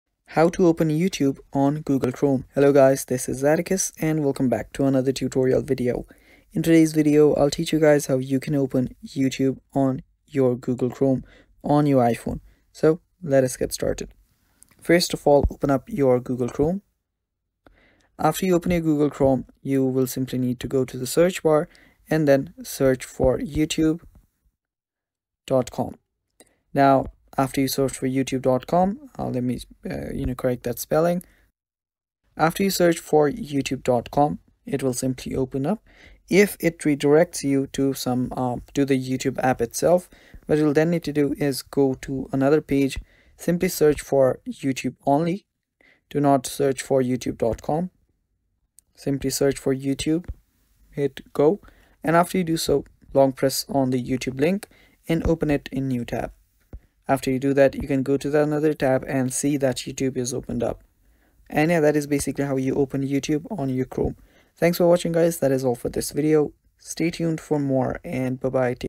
How to Open YouTube on Google Chrome
In this step-by-step tutorial, learn how to open YouTube on Google Chrome and start watching videos in just a few clicks. Whether you're using the YouTube website or the YouTube app in Chrome, this guide will show you how to access YouTube with ease.

In this video, you'll discover how to open YouTube in Google Chrome, how to watch YouTube in Chrome browser, and tips for using YouTube smoothly on Chrome.

Timestamps:
0:00 Introduction
0:20 Step 1: Opening Google Chrome
0:45 Step 2: Accessing YouTube on Chrome